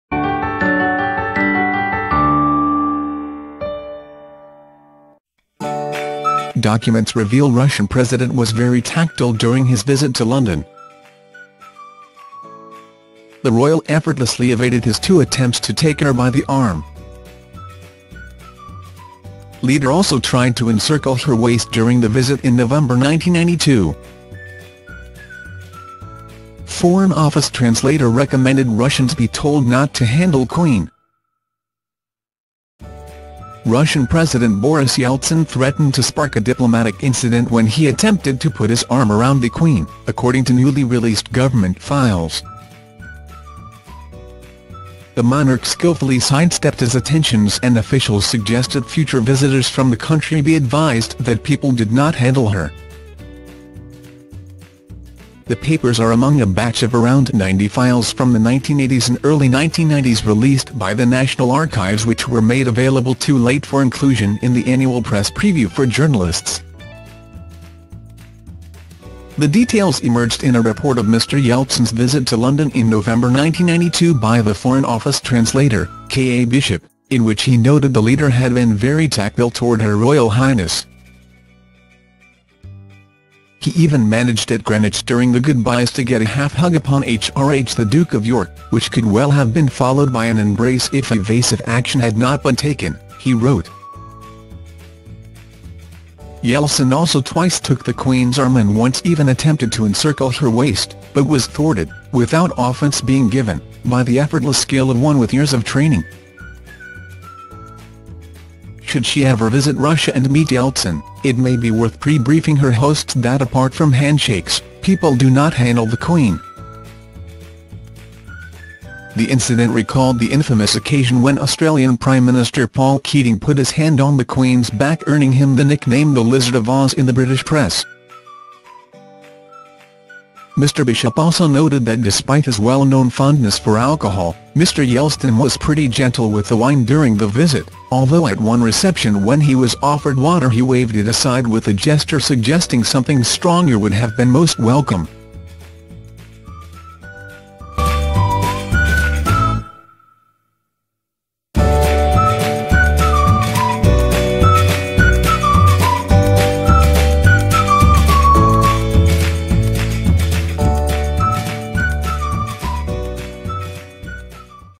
Boris Yeltsin tried to put his arm around the Queen Documents reveal Russian president was very
Documents reveal Russian president was very tactile during his visit to London The Royal effortlessly evaded his two attempts to take her by the arm Leader also tried to encircle her waist during the visit in November 1992 Foreign Office translator recommended Russians be told not to handle Queen Russian President Boris Yeltsin threatened to spark a diplomatic incident when he attempted to put his arm around the Queen according to newly released government files

Like any other engaged couple Pri Prince Harry and Meghan Markle DENIED their choice of reception :